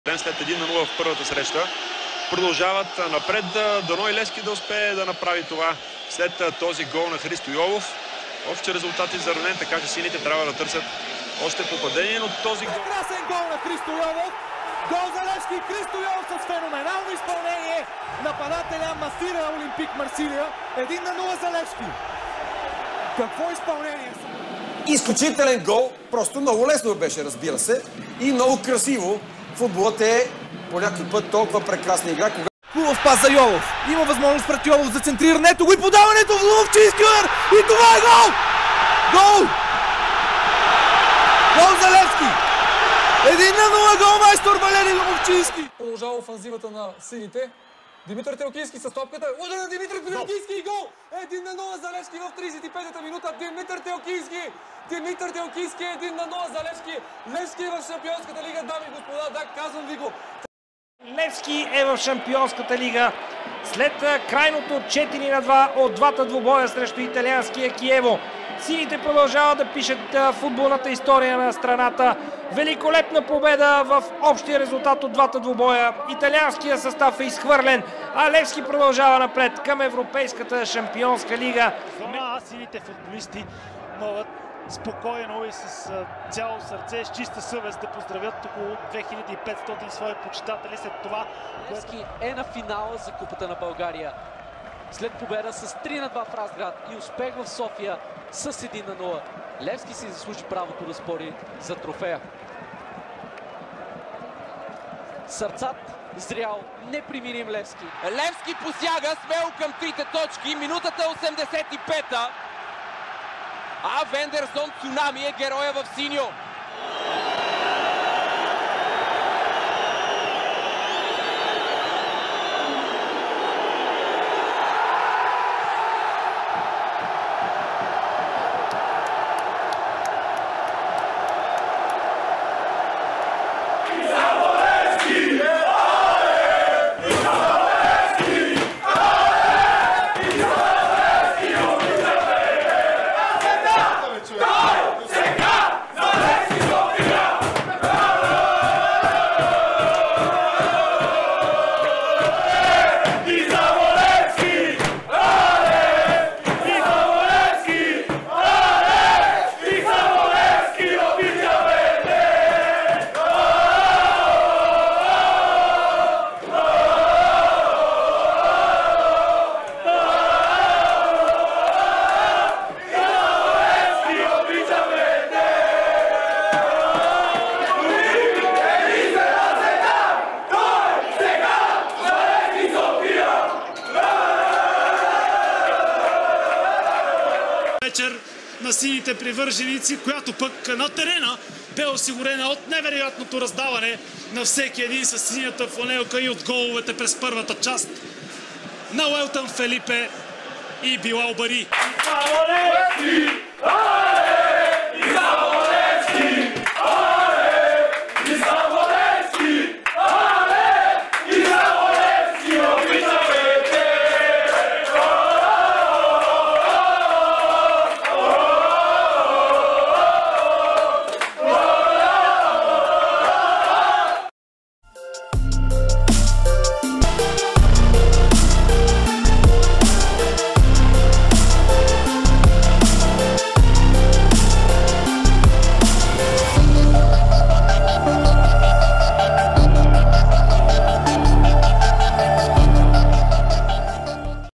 Stanimir Stoilov goal/win celebrations
Compilation of goal and win celebrations of the FC Levski Sofia head coach Stanimir Stoilov (+ some in FC Astana).
From the games against: NK Publikum Celje, Olympique de Marseille,  Schalke 04, A.C. Chievo Verona, Ludogorets, Cherno More, Litex-Sofia, PAOK and in FC Astana against APOEL Nicosia and Benfica.
Recordings from: BNT, BTV, Nova TV, Diema TV, Levski TV, FC Astana TV.